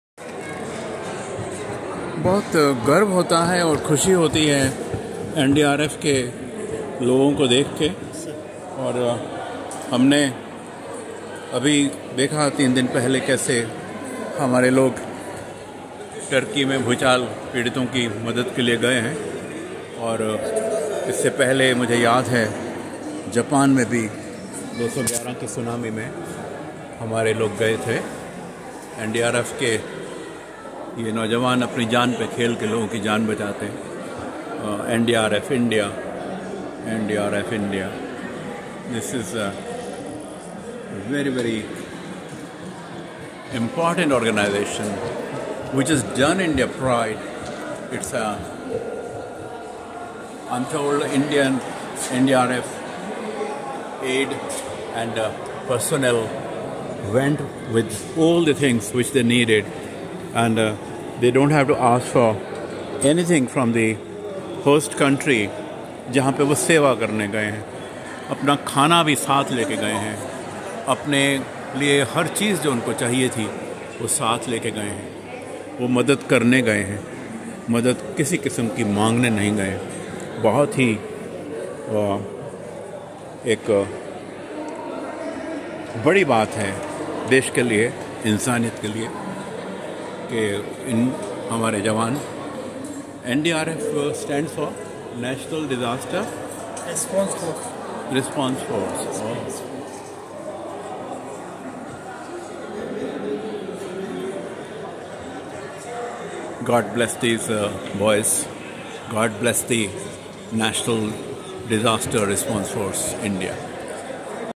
Turkey Quake Saviours: Indian NDRF
Now helping the QuakeVictimsTurkey

They helped in NepalQuake and